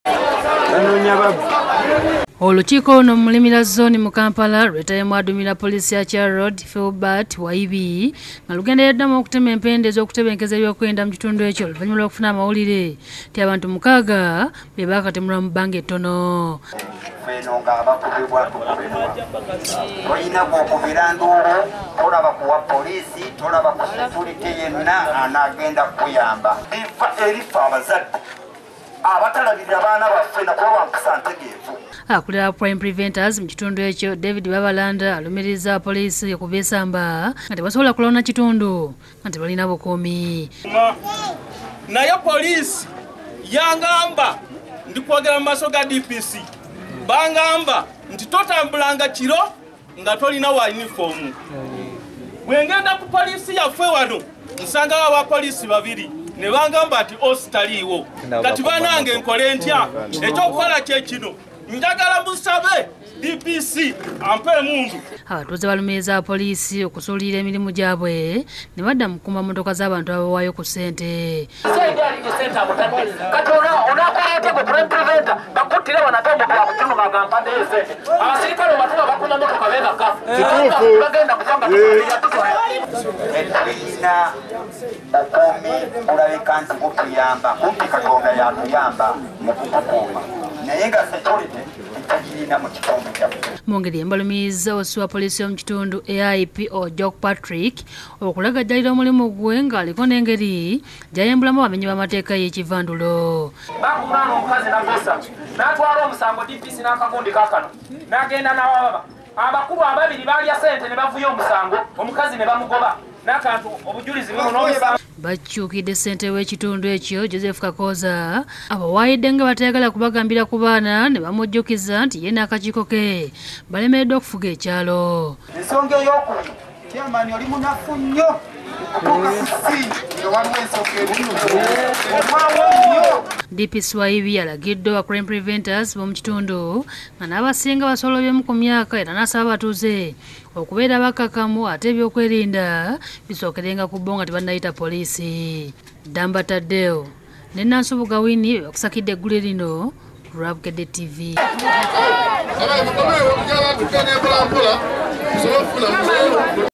Abatuuze balumirizza OC mu maaso ga DPC
Eby'okwerinda bimulemye okunyweza mu kitundu.